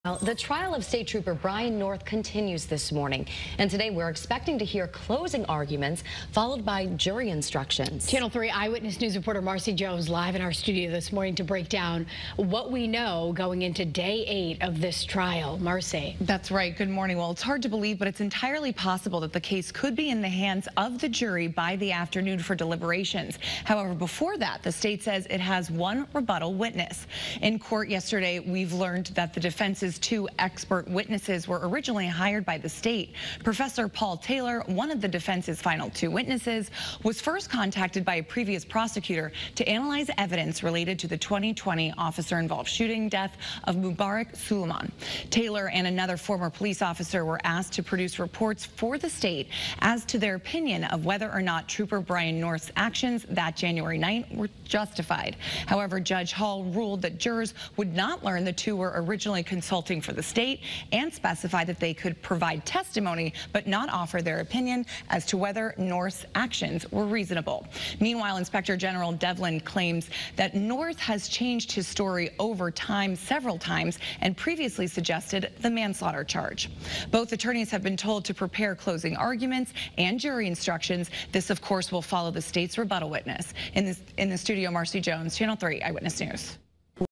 VIDEO: Trooper North closing arguments, jury instructions expected today
Closing arguments and jury instructions were expected on day 8 of the Trooper Brian North trial on Wednesday.